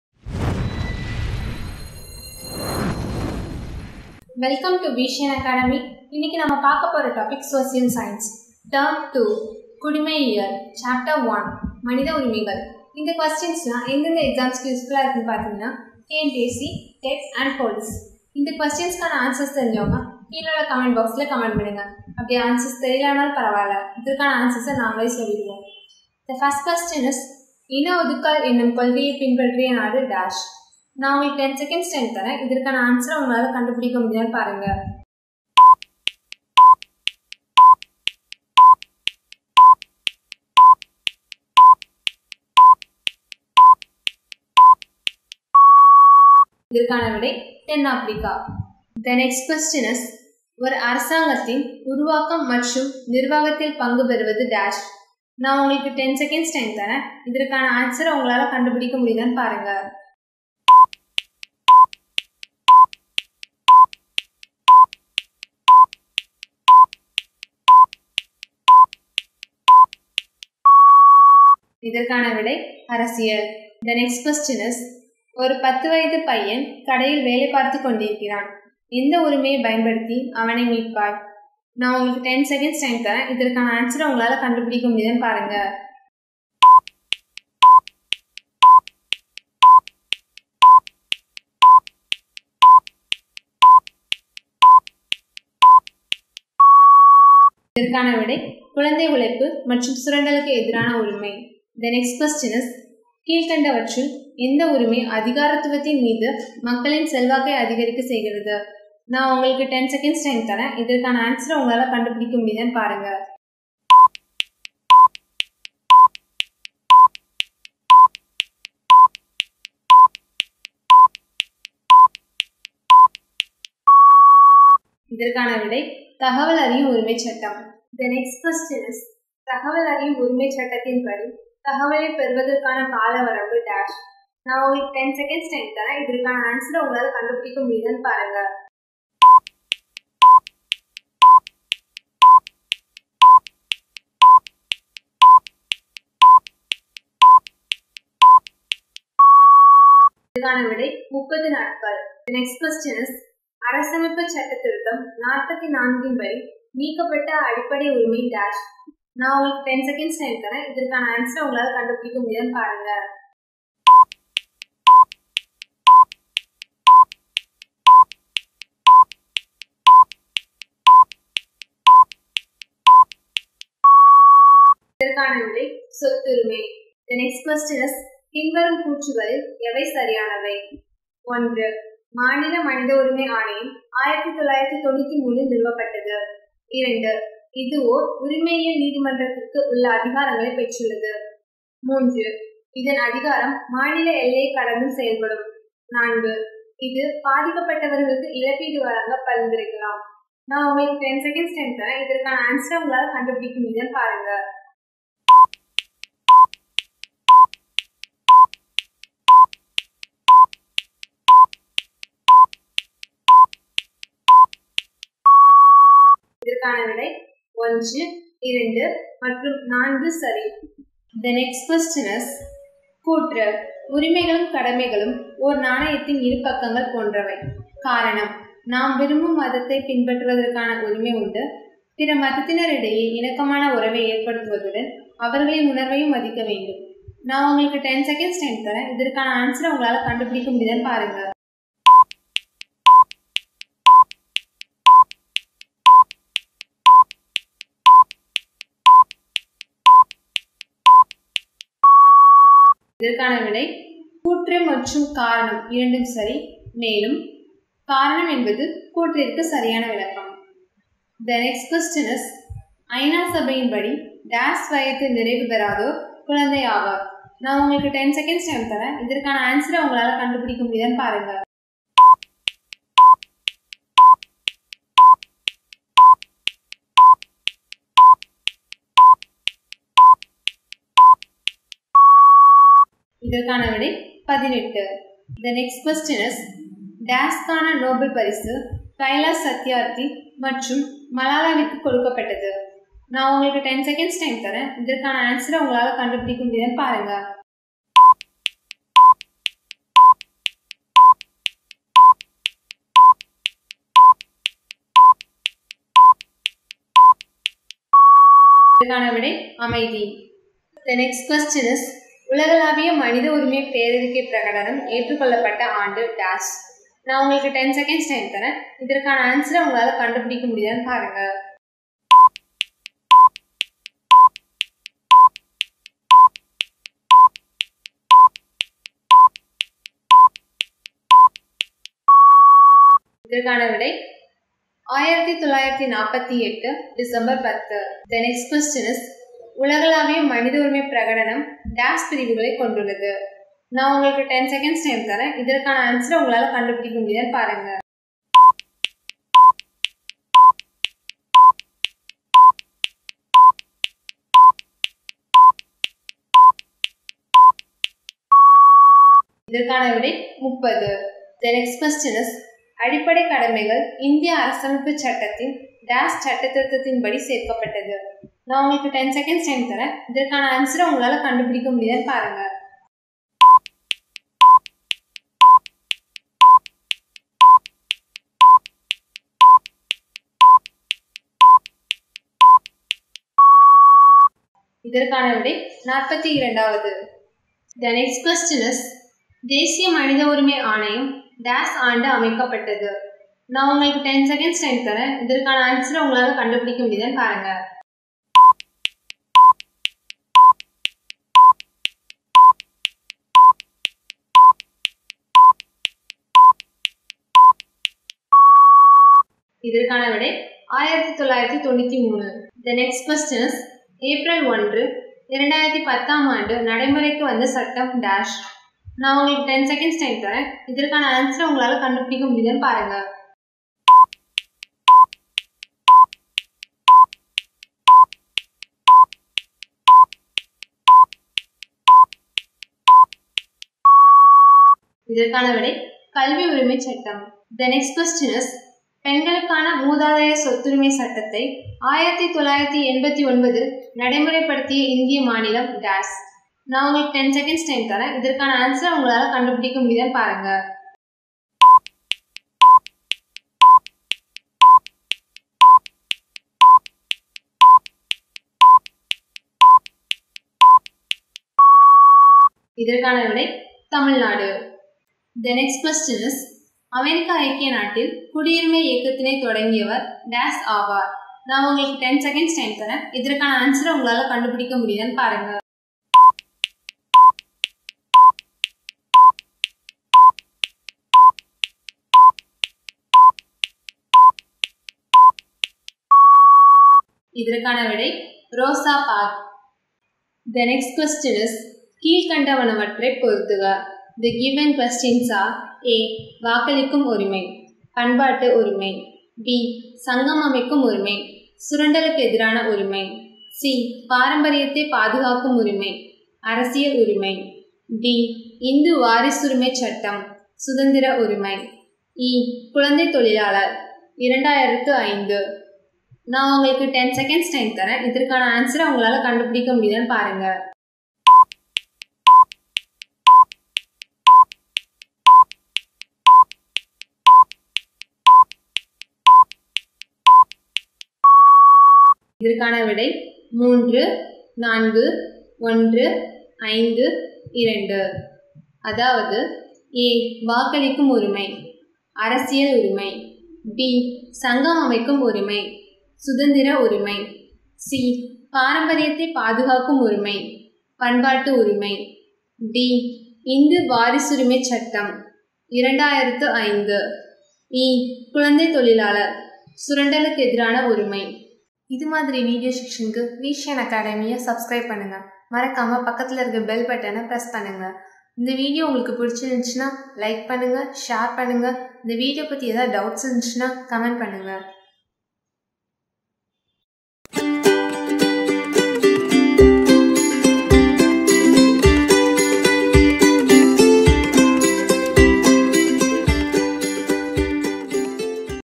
9th Std Social Science Model Questions Tamil Term 2 Chapter 7 | TNPSC, RRB, SSC | We Shine Academy
We Shine Academy Watch 9th Standard Social Science Model Questions Tamil Term 2 Chapter 7, from the Experts of Best TNPSC Coaching Centre in Chennai.

notification/

Category